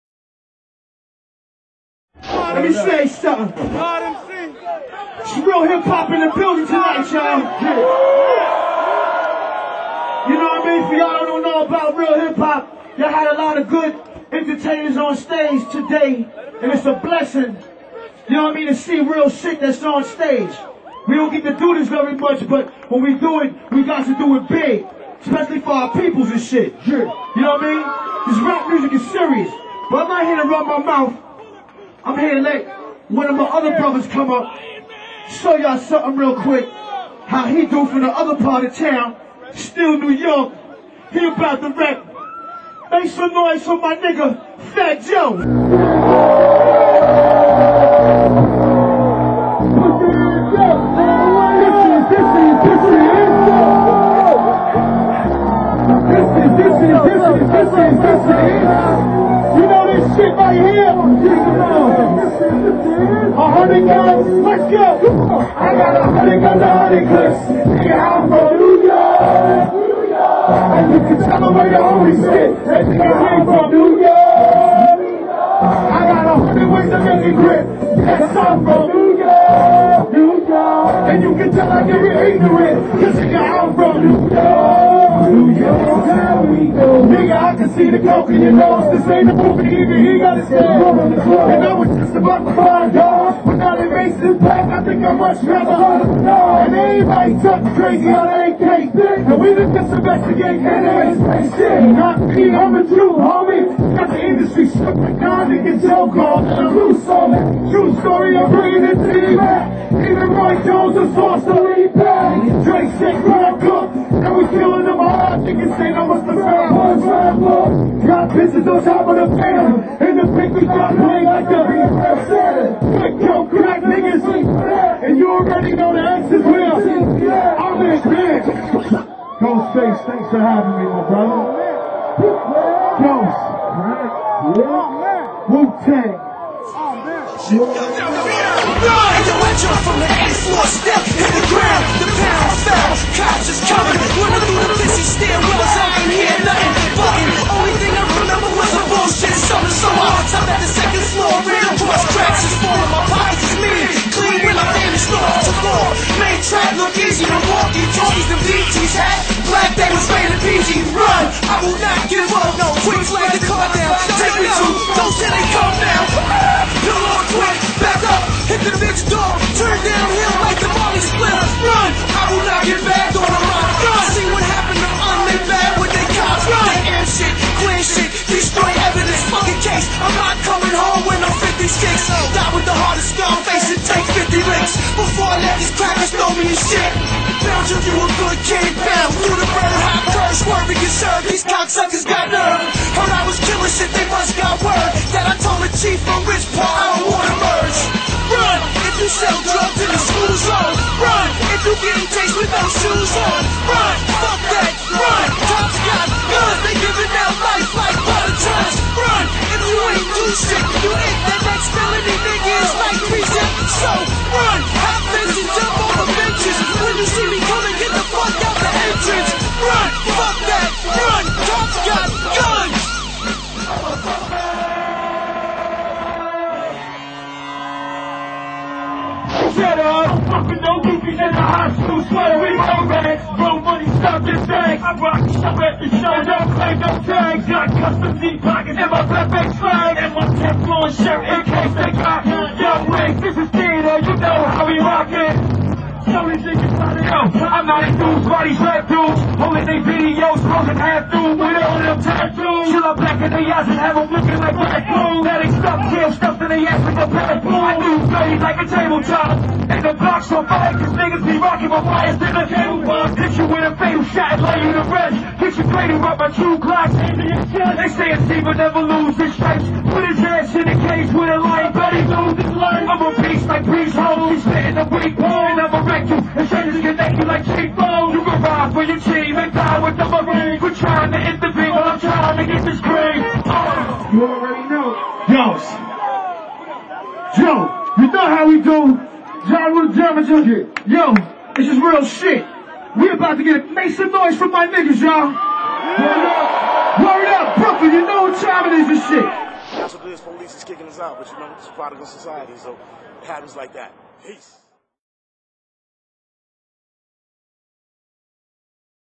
Ghostface, FatJoe & Trife da God
Ghostface Kills it at the Brooklyn Hip Hop Festival and He Brought some Friends Along!